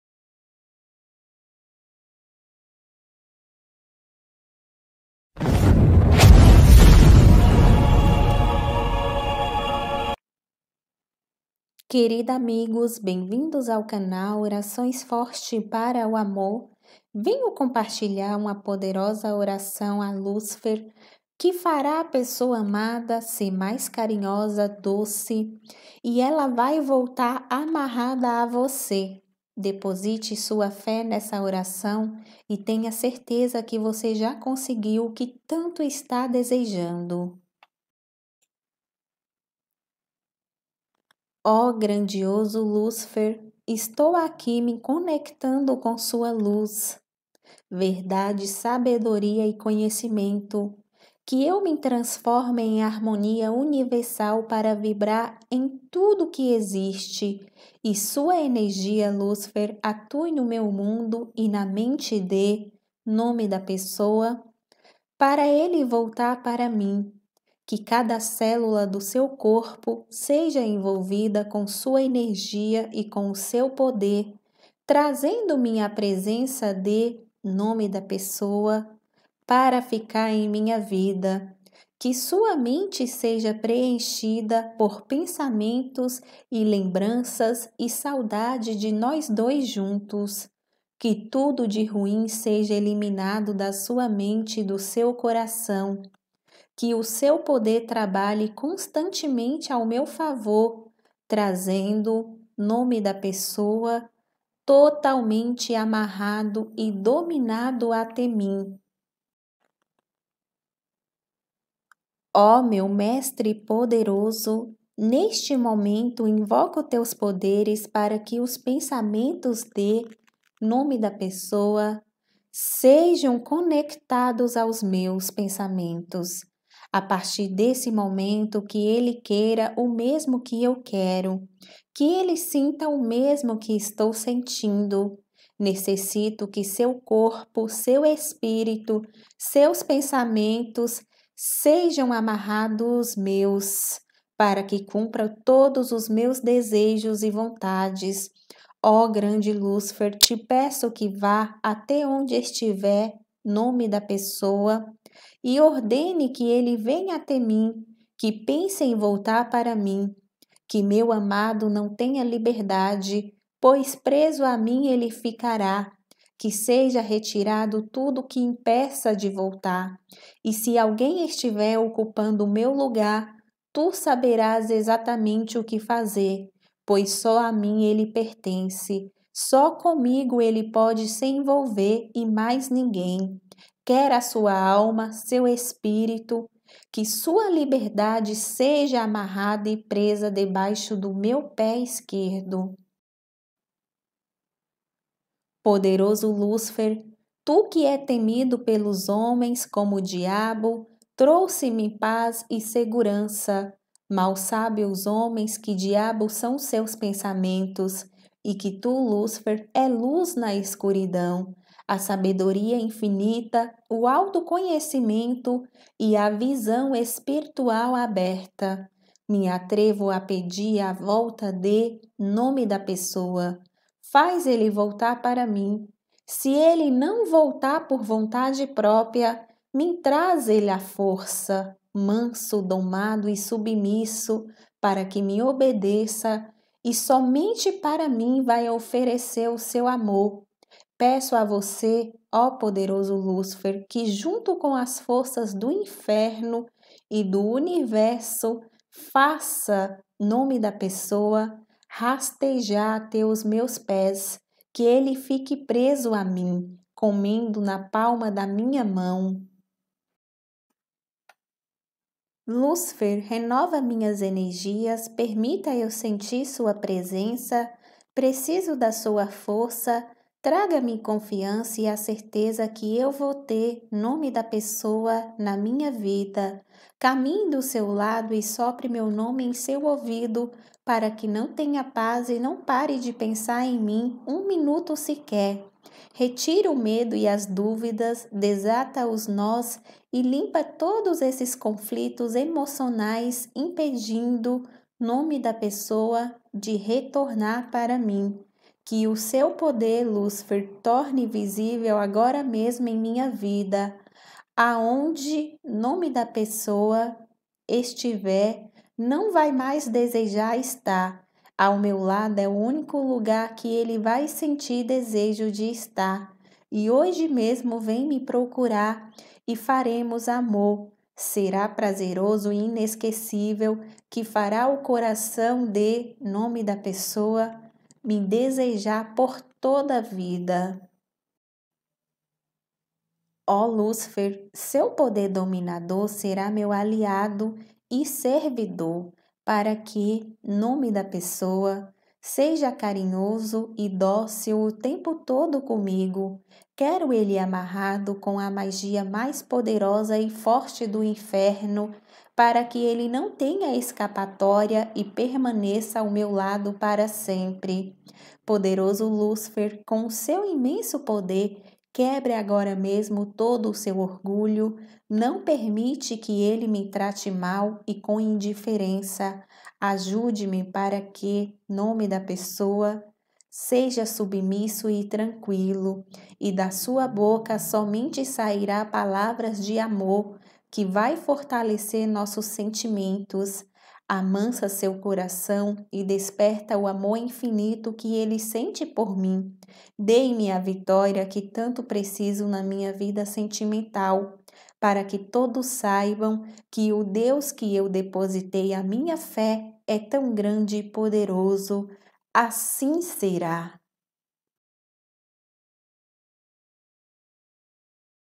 Oração a Lúcifer para amarrar e trazer de volta o amor da sua vida.
Oração muito forte para ele sentir T£SÃO por você Deixa a pessoa com muita vontade de você.